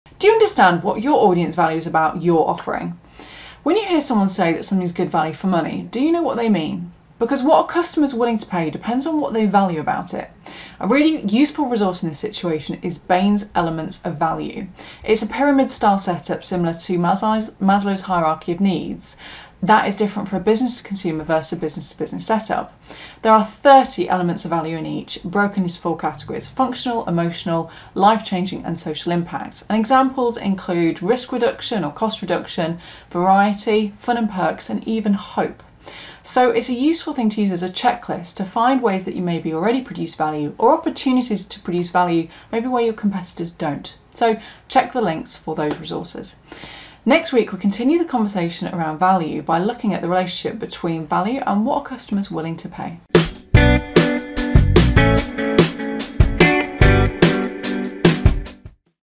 Do you understand what your audience values about you?
This week's Ideal Marketing Minute video explores the crucial concept of value in business and how understanding what your audience values can enhance your offerings. We'll dive into a powerful framework that categorises the various elements influencing consumer perception and decision-making. By analysing these elements, you'll learn how to identify gaps in your offerings and create compelling value propositions that set you apart from competitors.